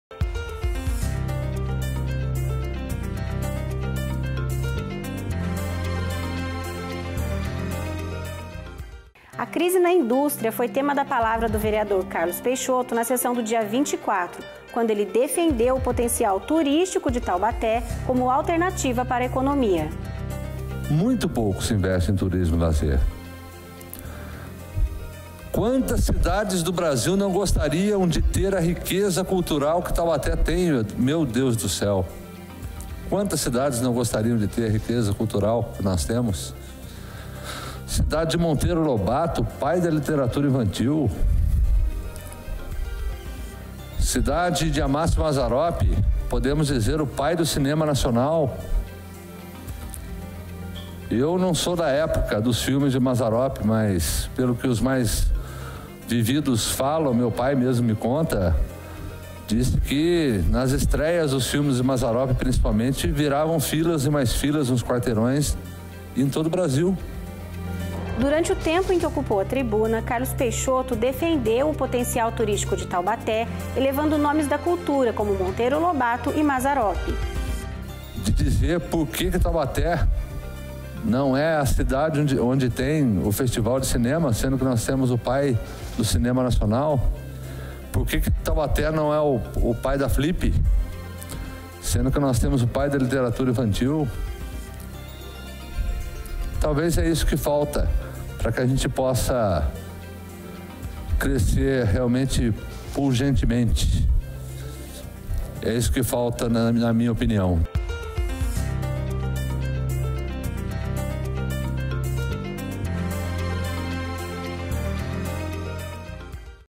Minuto Legislativo - Ver. Carlos Peixoto - 163ª Sessão Ordinária - 24/08/2015
A crise na indústria foi tema da palavra do vereador Carlos Peixoto na sessão dia 24, quando ele defendeu o potencial turístico de Taubaté como alternativa para a economia.